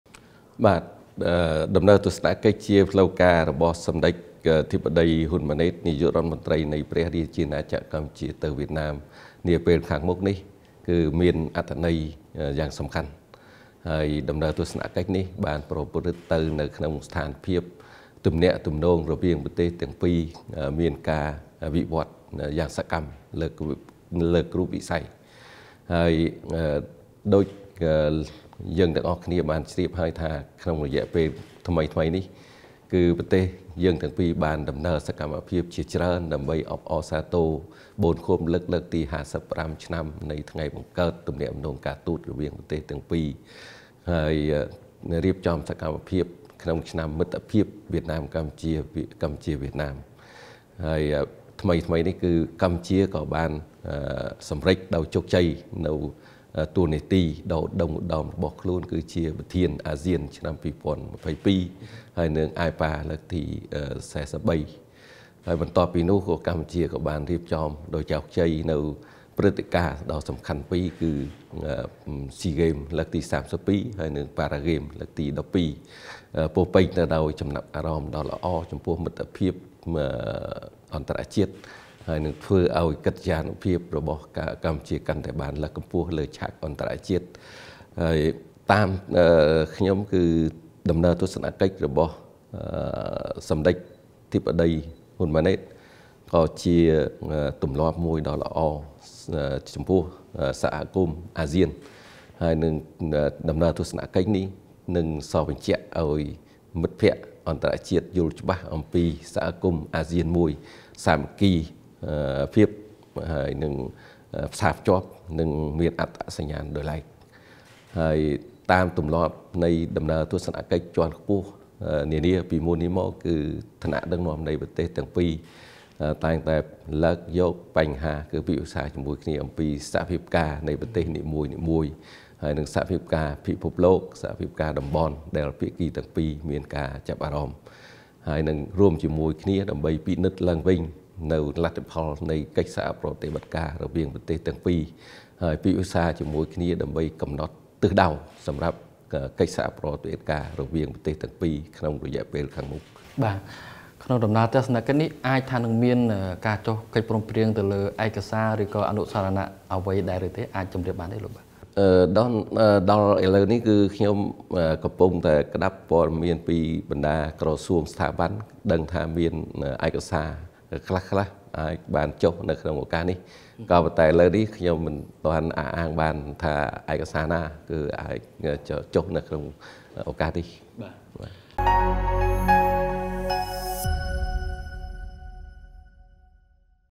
ដំណើរទស្សនកិច្ចរបស់សម្តេច ហ៊ុន ម៉ាណែត ទៅប្រទេសវៀតណាមនាពេលខាងមុខនេះ មានអត្ថន័យយ៉ាងសំខាន់
លោក Nguyen Huy Tang ឯកអគ្គរដ្ឋទូតវៀតណាមប្រចាំកម្ពុជាអះអាងថា ដំណើរទស្សនកិច្ចរបស់សម្តេចនាយករដ្ឋមន្ត្រី ហ៊ុន ម៉ាណែត ទៅប្រទេសវៀតណាមនាពេលខាងមុខនេះ មានអត្ថន័យយ៉ាងសំខាន់ និងប្រព្រឹត្តទៅក្នុងស្ថានភាពទំនាក់ទំនងរវាងប្រទេសទាំងពីរមានការវិវឌ្ឍយ៉ាងសកម្មលើគ្រប់វិស័យ